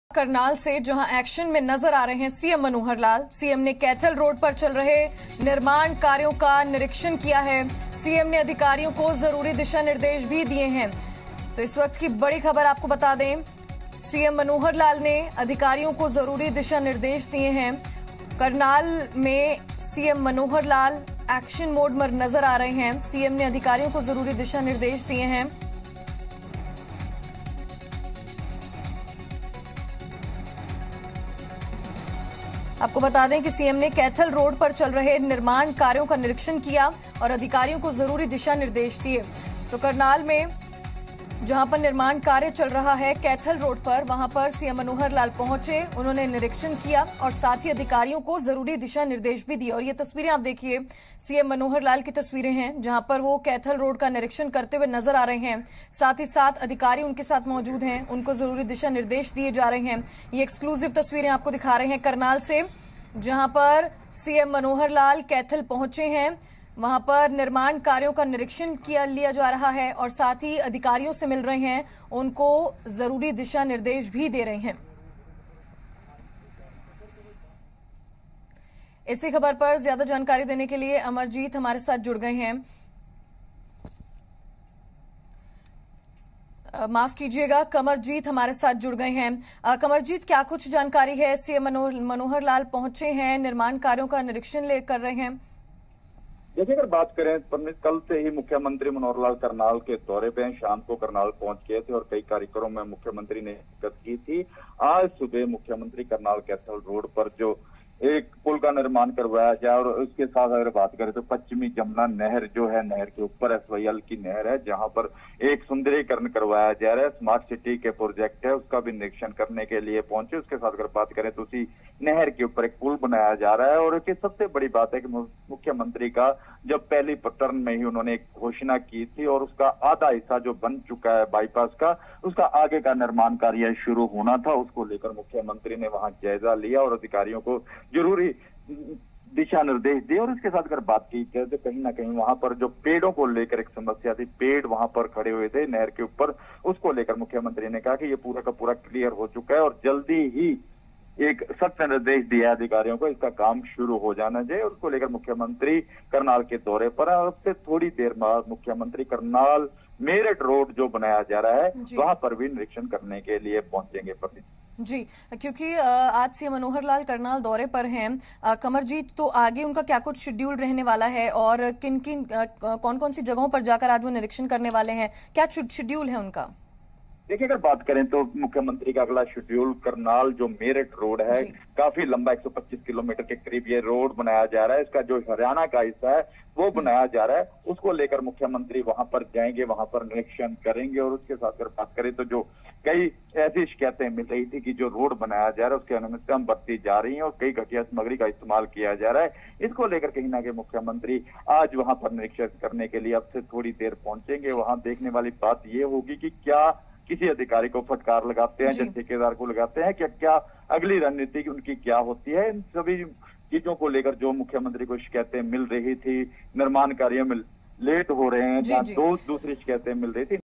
Haryana Latest News:CM Manohar Lal ने Kaithal Road पर चल रहे निर्माण कार्यों का किया निरीक्षण ZeePHH
सीएम मनोहर लाल ने कैथल रोड पर चल रहे निर्माण कार्यों का किया निरीक्षण